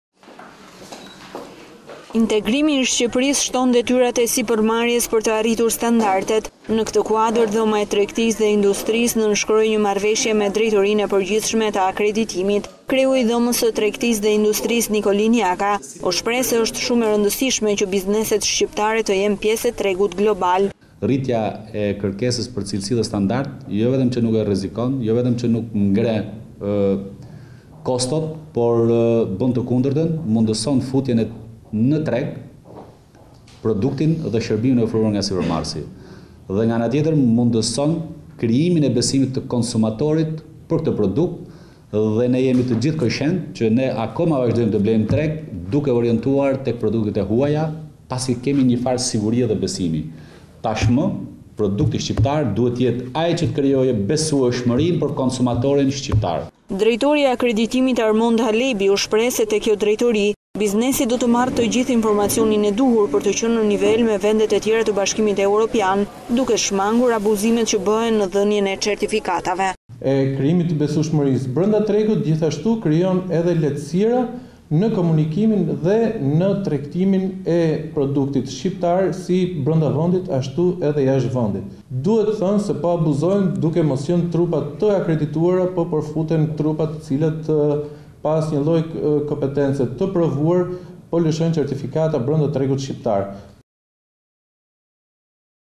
Standarde të reja për sipërmarrjen- RTV Ora News- Lajmi i fundit-
Dhoma e Tregtisë dhe Industrisë në Tiranë dhe Drejtoria e Përgjithshme e Akreditimit nënshkruajnë marrëvëshje bashkëpunimi.
Integrimi i Shqipërisë shton detyrat e Sipërmarrjes për të arritur standardet. Në  këtë kuadër Dhoma e Tregtisë dhe Industrisë nënshkroi një marrëveshje me drejtorinë e Përgjithshme të Akreditimit.
Kreu i Dhomës  së Tregtisë dhe Industrisë Nikolin Jaka u shpreh se është shumë e rëndsishme që bizneset shqiptare të jenë pjesë e tregut global.